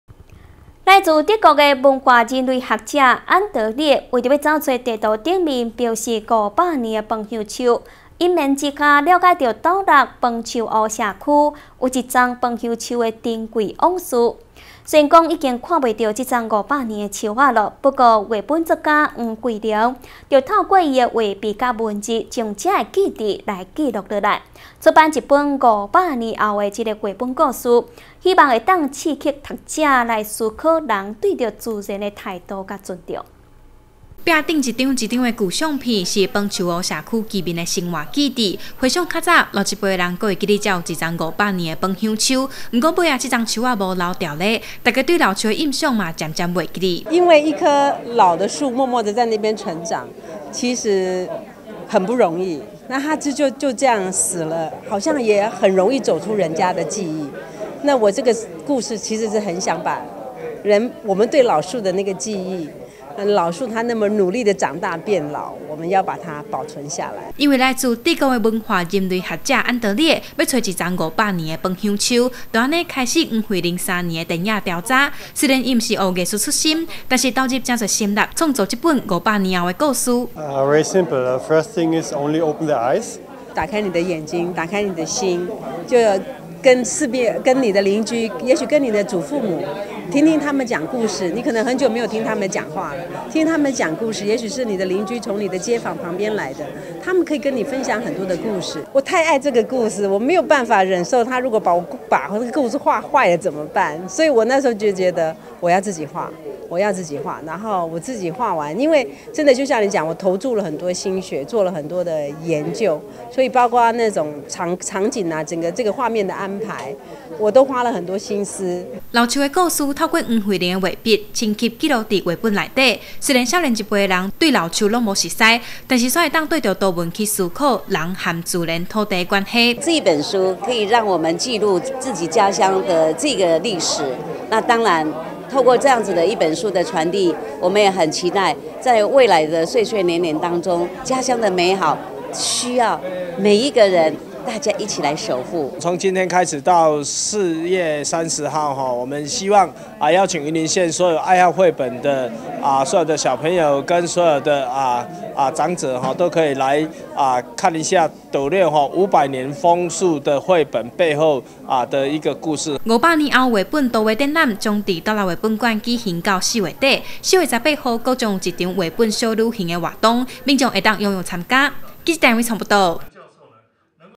雲林新聞網-"五百年後"繪本 重現楓香樹珍貴往事
來自德國的文化人類學者安德烈，為了找尋台灣地圖上標示的五百年楓香樹，因緣之下了解斗六楓樹湖社區有一棵楓香樹的珍貴往事，雖然已看不到這棵五百年楓香樹，但繪本作家黃惠玲透過圖文，將這些記憶記錄下來，出版一本《五百年後》的繪本故事，希望刺激讀者思考，人類對於自然的態度與尊重。〈以上新聞可從top NEWS APP收視〉